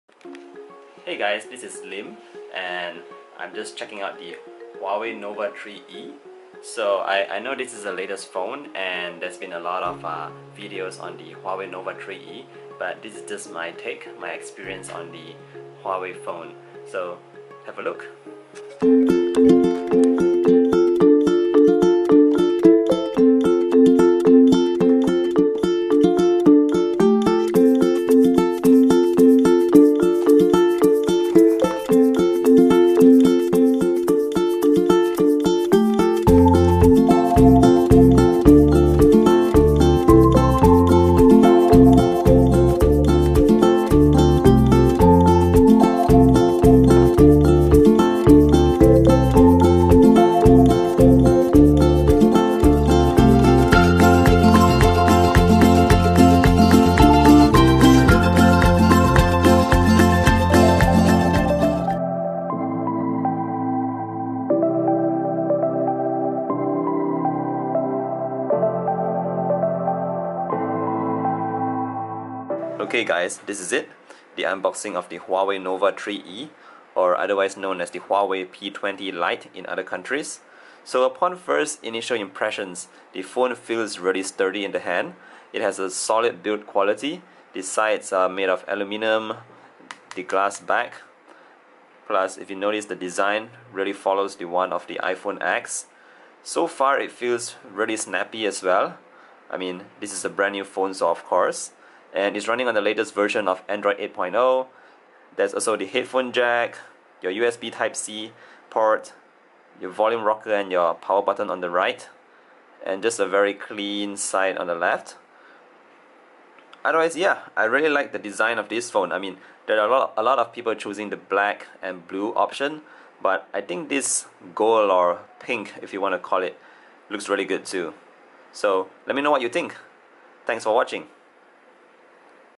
Huawei Nova 3e in Sakura Pink! Unboxing and First Impressions!
My very first ever YouTube video!! Please be kind :)
I really liked this new budget phone.
It costs RMB2000 here in China.
That will be approx:
• USD320.00
• INR20,500.00
• EUR258.00